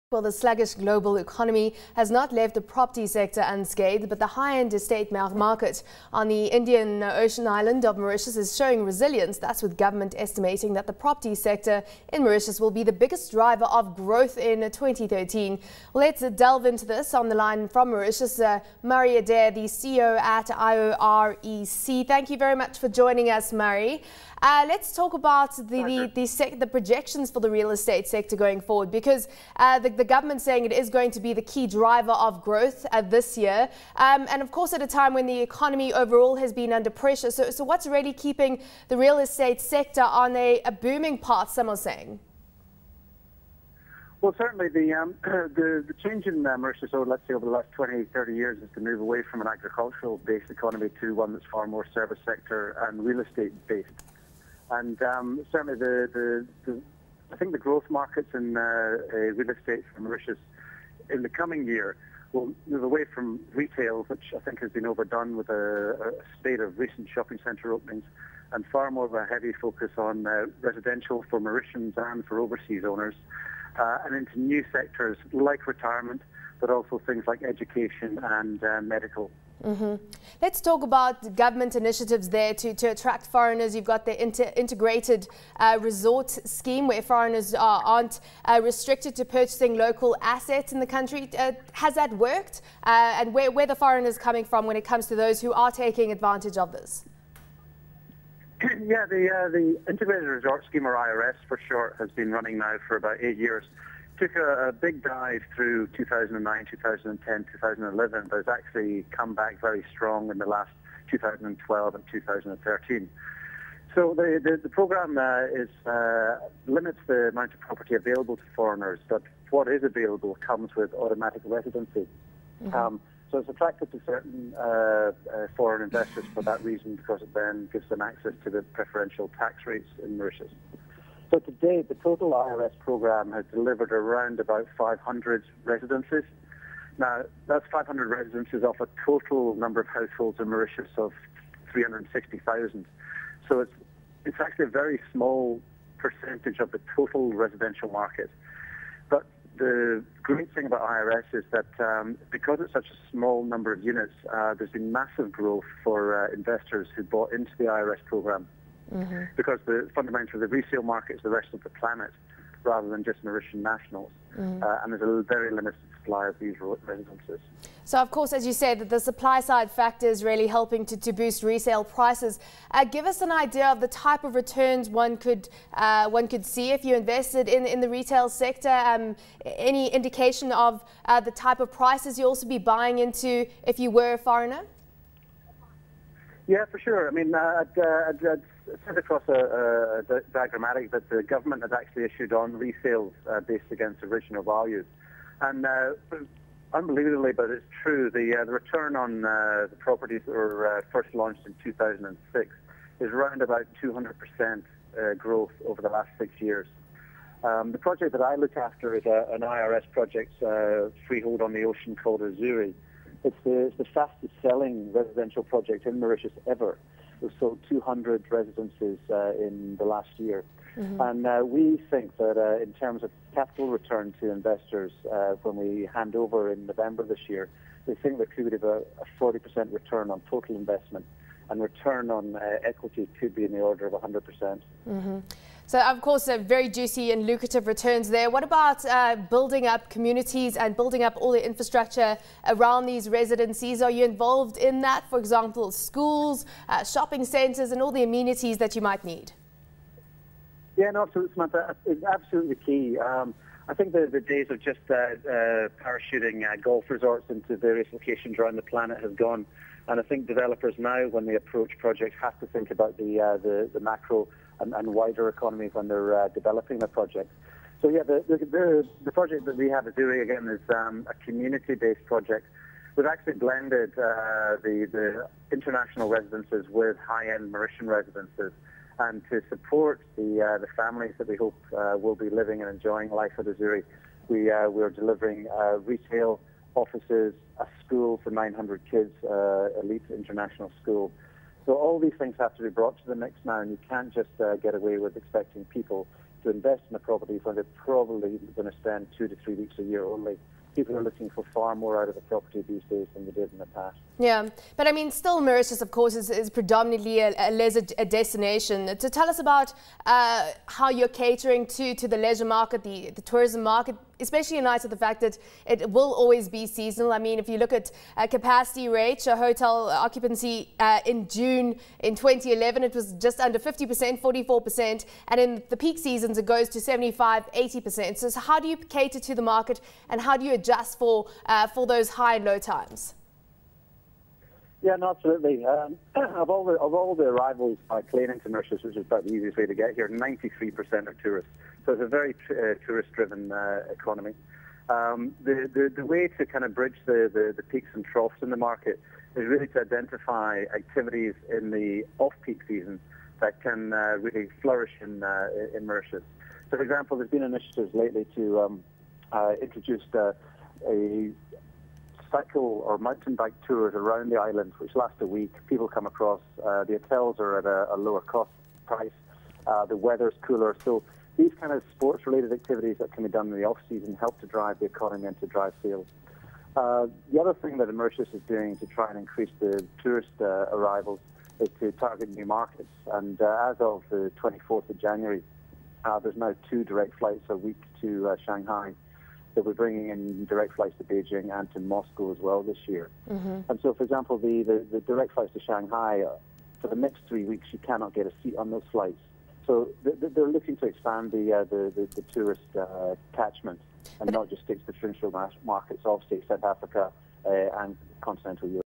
Mauritius Real Estate Sector with Murray Adair
The sluggish global economy has not left the property sector unscathed, but the high-end estate market on the Indian Ocean island of Mauritius is showing resilience. ABN's Samantha Loring speaks with Murray Adair, CEO of IOREC, in Mauritius.